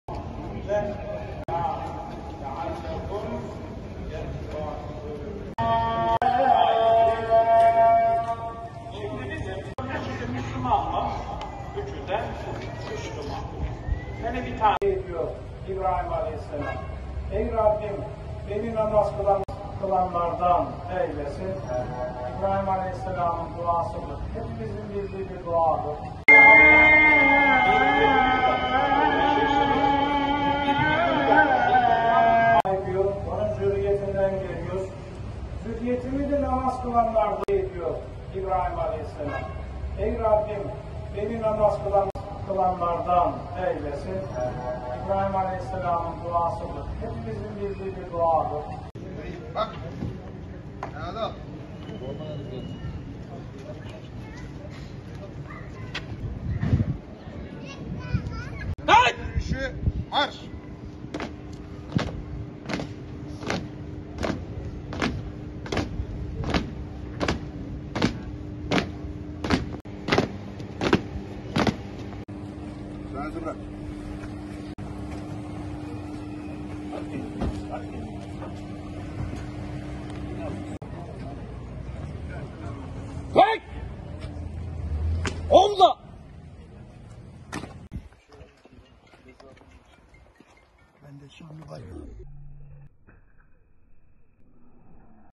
Eski Pamukova Astsubaylar Derneği Başkanı Askeri törenle defnedildi
Eski TEMAD Pamukova Şube Başkanı ve Astsubaylar Derneği Başkanı Nami Özen askeri törenle son yolculuğuna uğurlandı